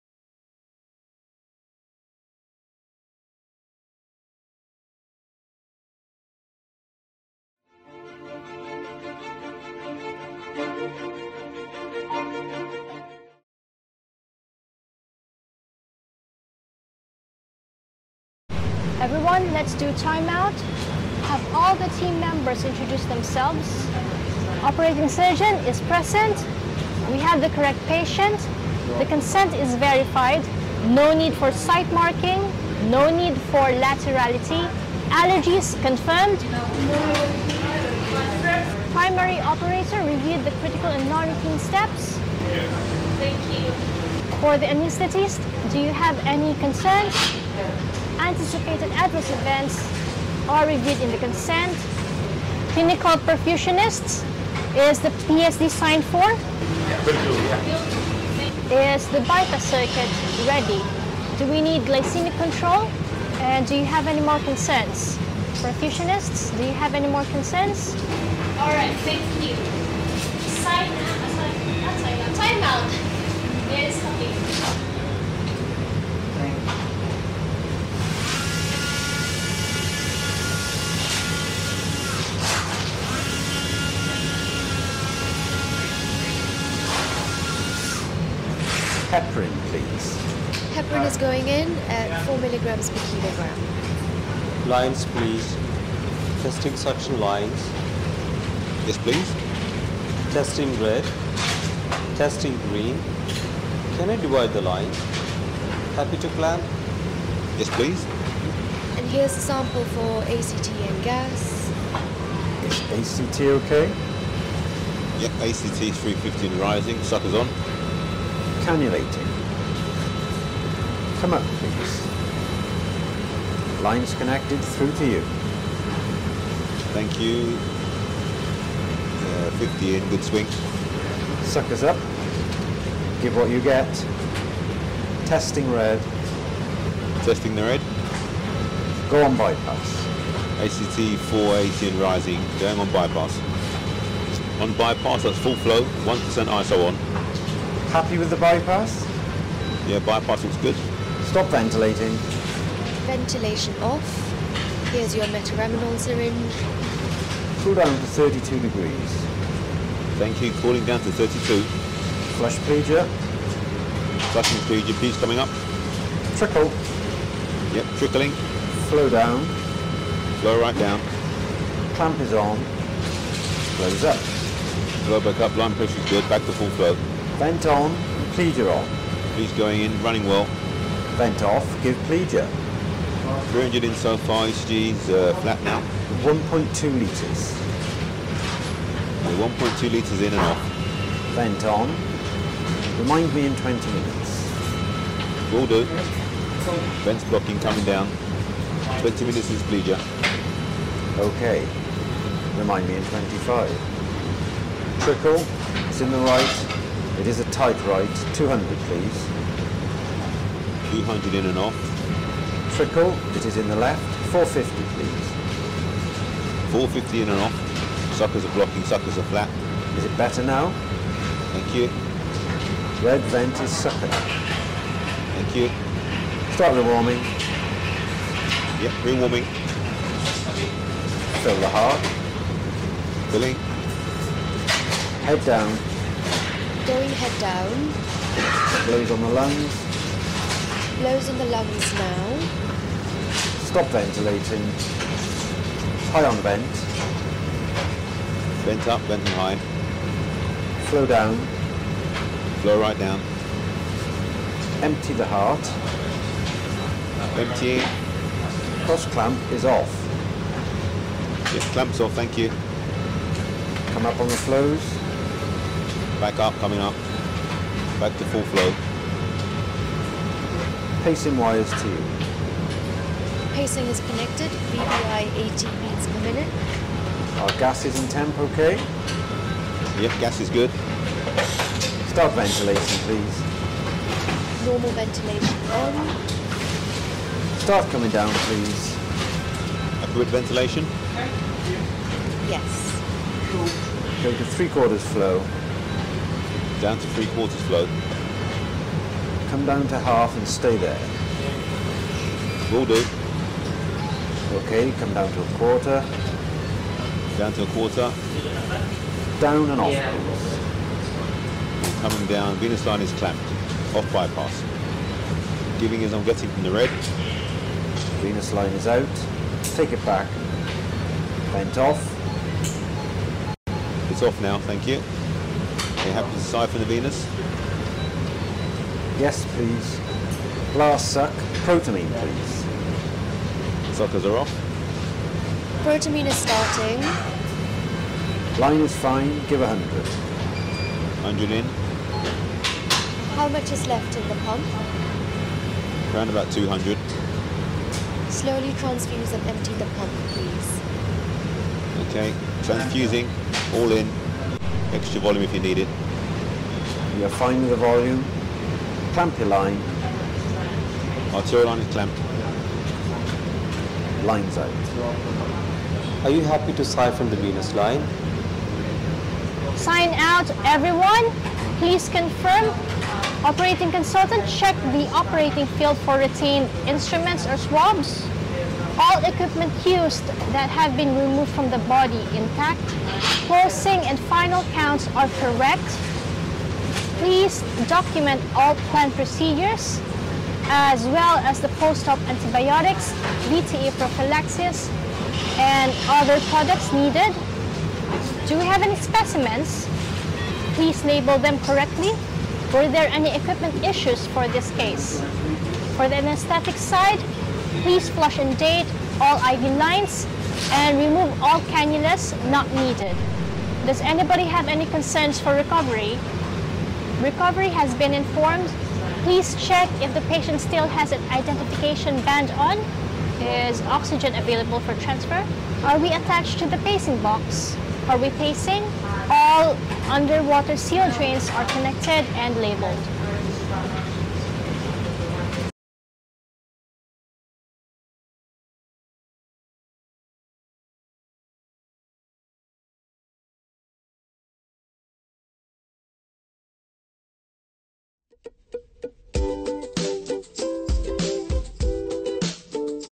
Brief Stroll Through Cardiac Surgery
Cardiac Surgery is a very complicated procedure  which  can take many hours .
 In this video we are taking a brief  stroll through the procedure which is condensed to 7 min.
Michael Whitehorne